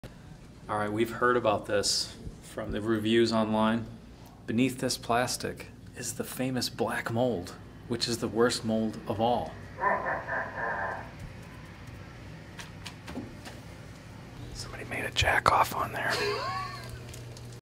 Black Mold or Bodily Fluids - Which is Worse?
Nothing but filth on every surface.

…from the ill-fated trip to the 2016 Indianapolis Kiss Expo at the Caribbean Cove.